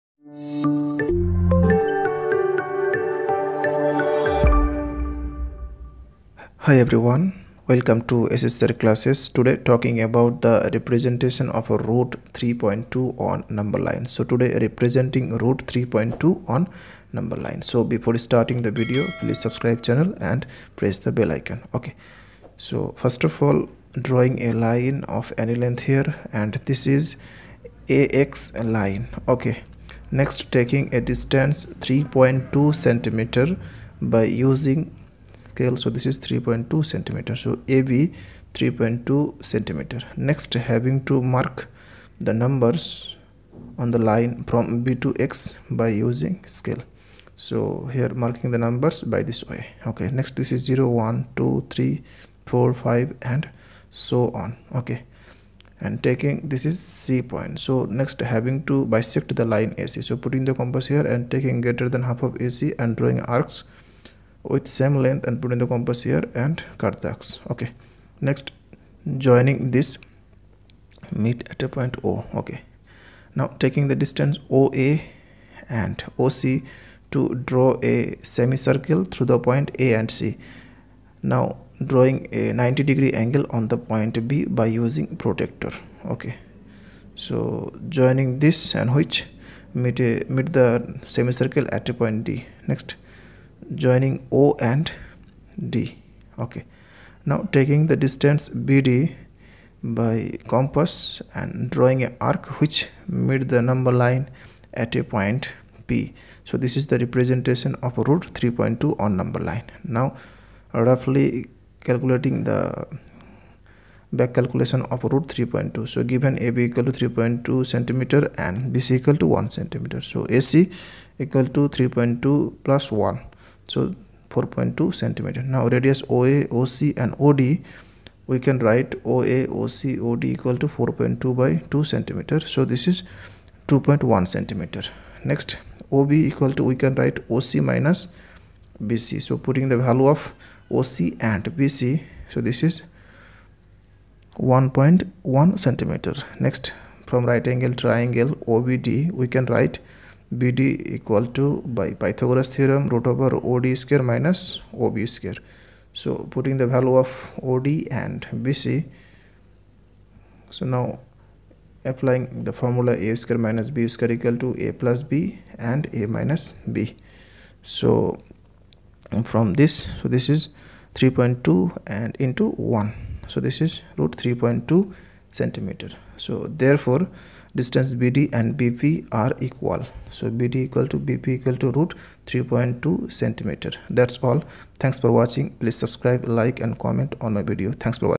How to represent root 3.2 on number line. shsirclasses.
In Easy Way how to represent root 3.2 on number line. Locate root 3.2 on number line. Geometrical Construction.